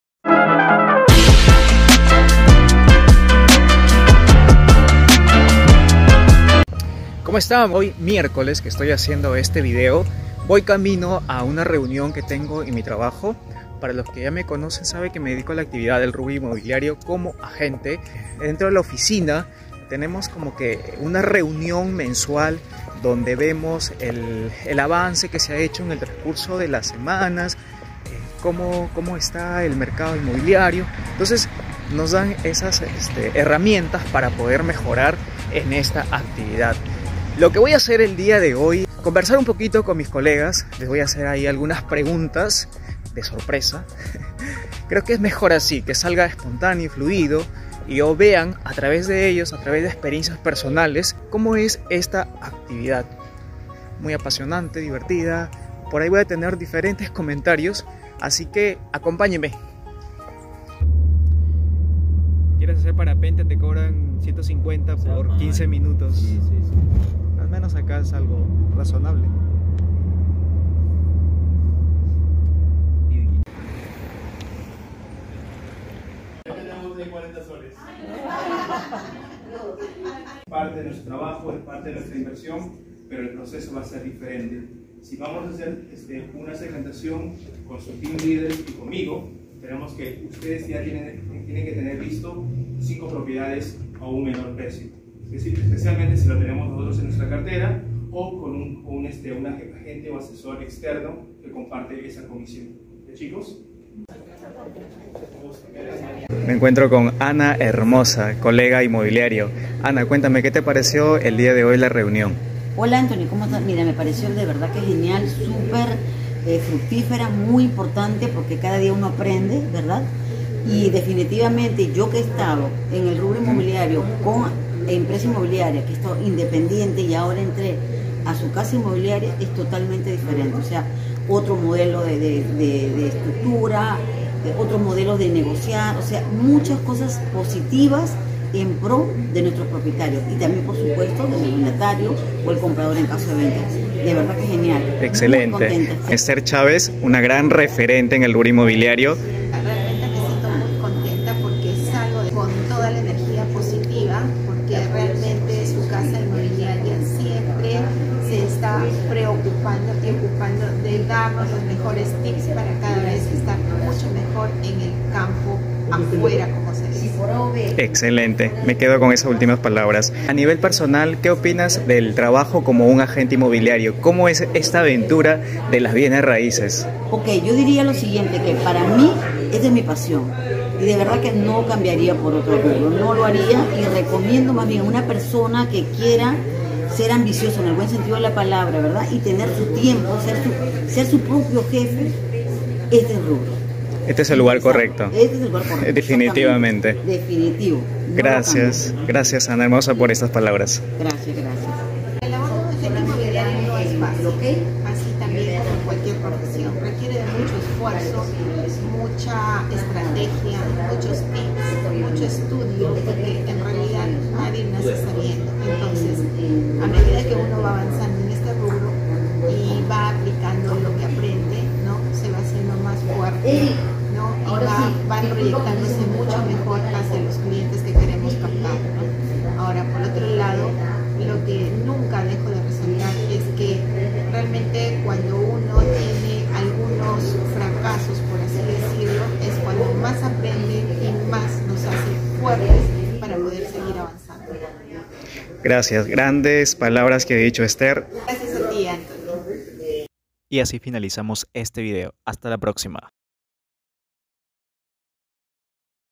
SER AGENTE INMOBILIARIO NO ES UNA TAREA FÁCIL🤔
Esta vez les muestro otra parte de mi  trabajo.
Ser agente inmobiliario al principio no es fácil, como toda profesión demanda sacrificio, perseverancia, capacitación, etc.
Recojo dos versiones de mis colegas con mas años en el rubro el cual nos comparten su experiencia.

Deja tu comentario o emoji 😉👉

También me encuentras en tiktok🤳